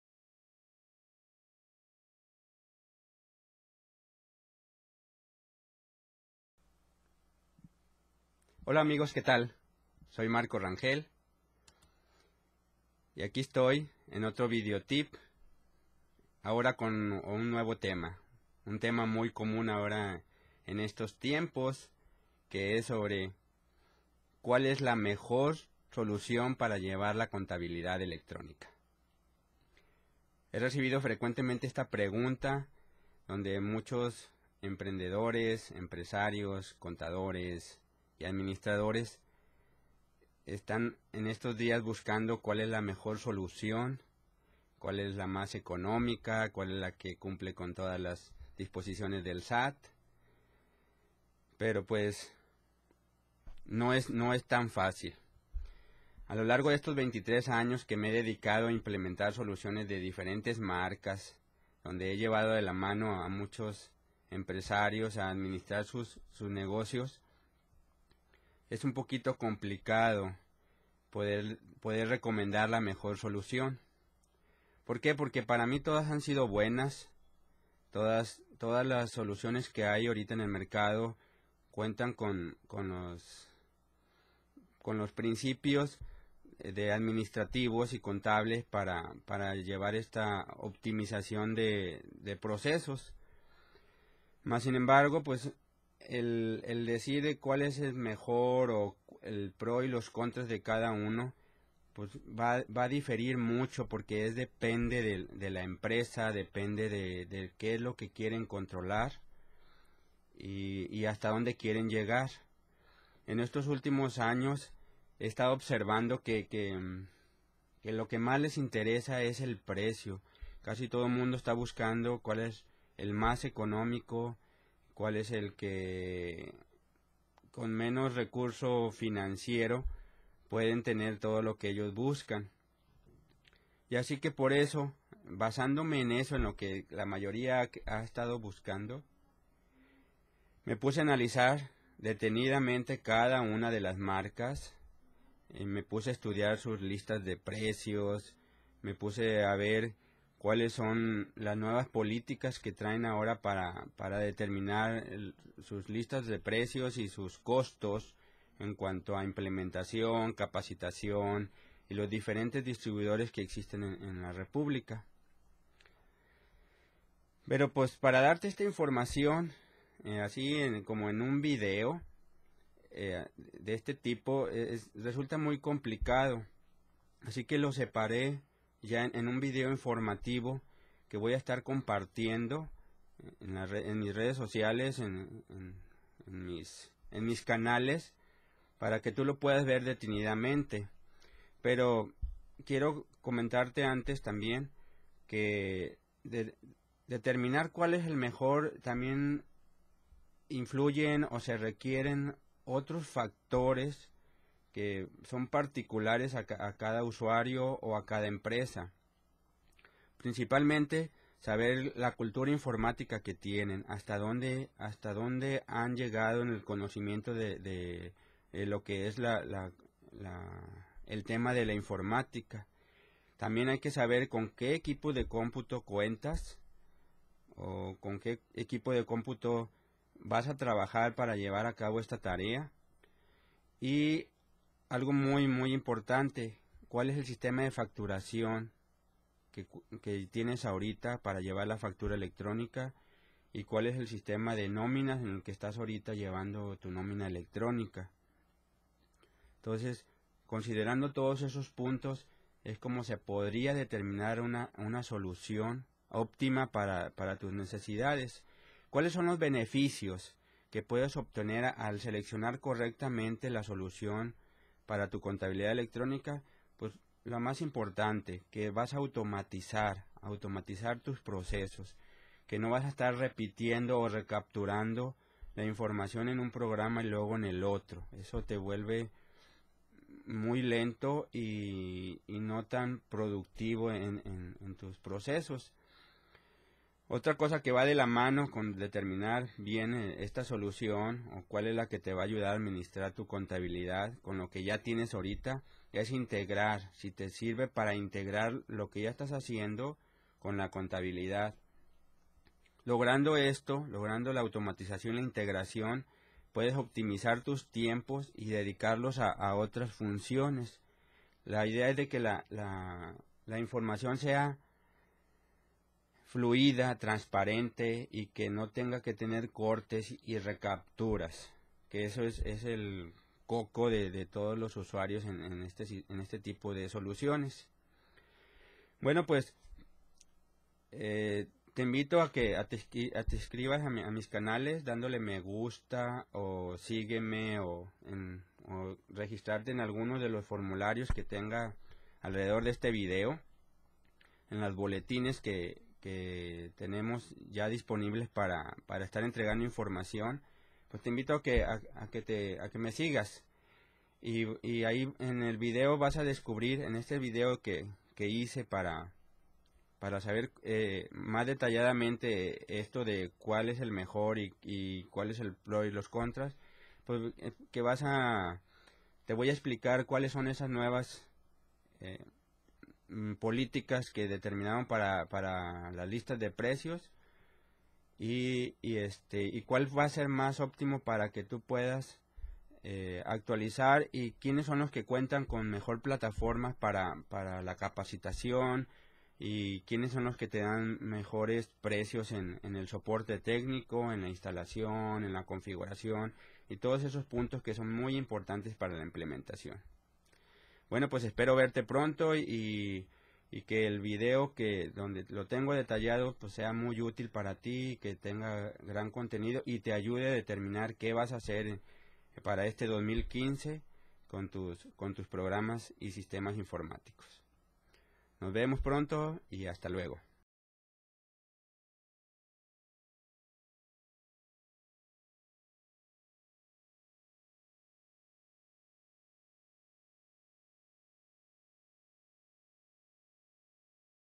¿Cual es la mejor solución para la Contabilidad Electrónica?
- Descubre cual es el mejor programa que hay en el mercado para manejar la Contabilidad Electrónica.